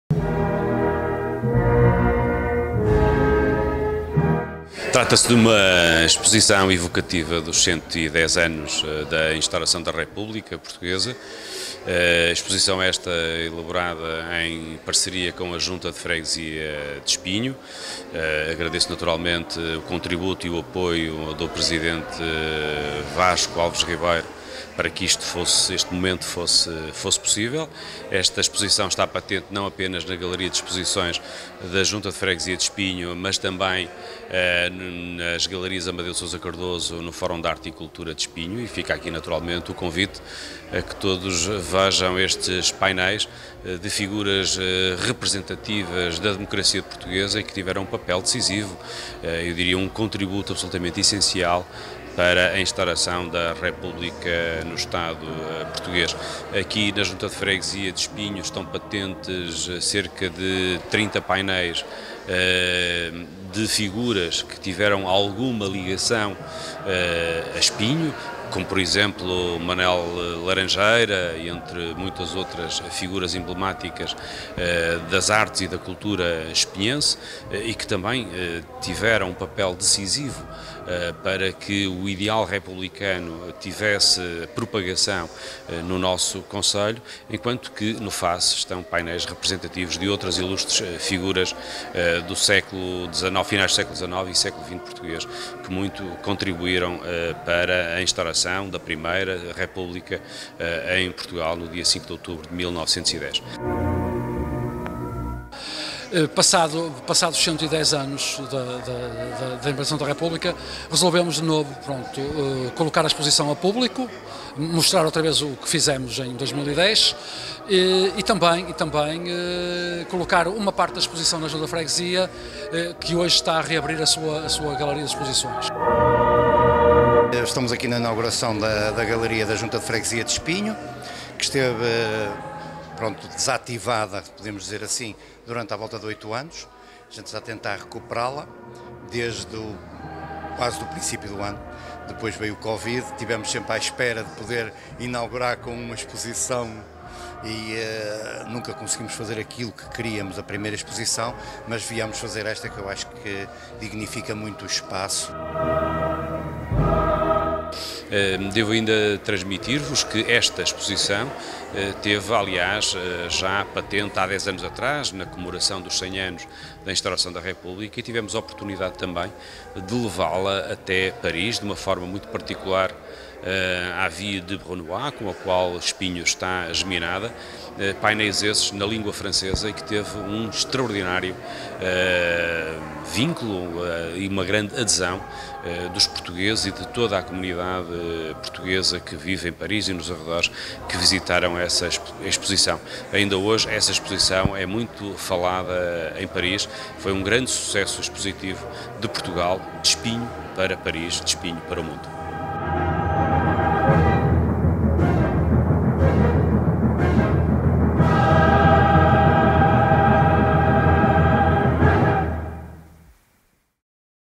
Inauguração da exposição "Rostos da República"
Esta segunda-feira, foi inaugurada na Junta de Freguesia de Espinho e Museu Municipal de Espinho, a exposição "Rostos da República". Uma exposição produzida pelo Museu Municipal de Espinho para as comemorações do centenário da implantação da República, em 2010.
A exposição estará patente até dia 31 de Outubro nestes dois espaços.